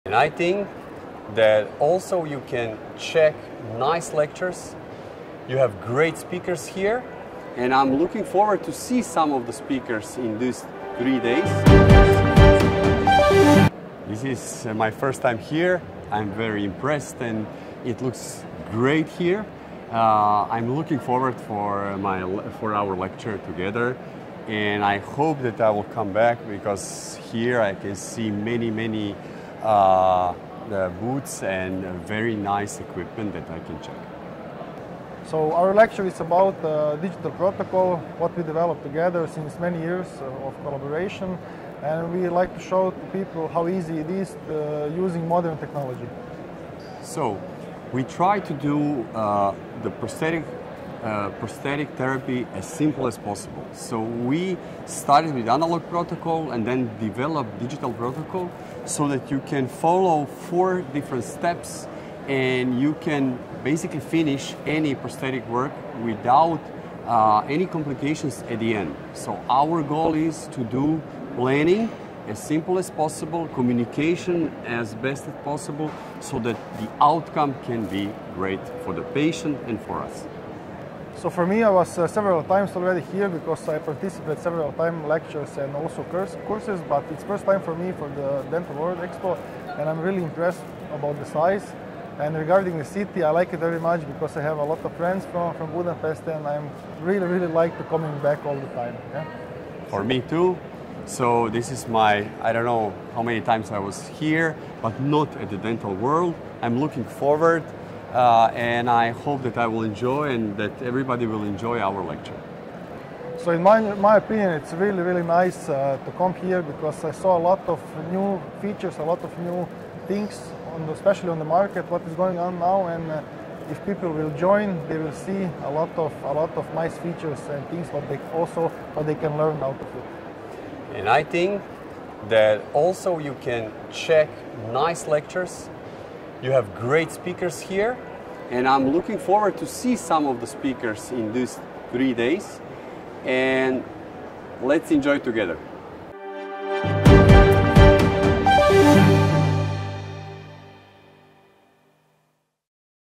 DR. MARKO JAKOVAC, DRAGAN STOLICA - Speakers of Aesthetics Congress | DentalWorld
Dr. Marko Jakovac and Dragan Stolica share the sentiment that the conference offers a valuable opportunity for attendees to learn from excellent lectures, hear from great speakers, and discover a multitude of new features and market insights.
Dr. Marko Jakovac and Dragan Stolica úgy gondolja, hogy a konferencia értékes lehetőséget kínál a résztvevők számára, hogy kiváló előadásokból tanuljanak, kiemelkedő előadókat hallhassanak, és a piaci újdonságok sokaságát fedezzék fel.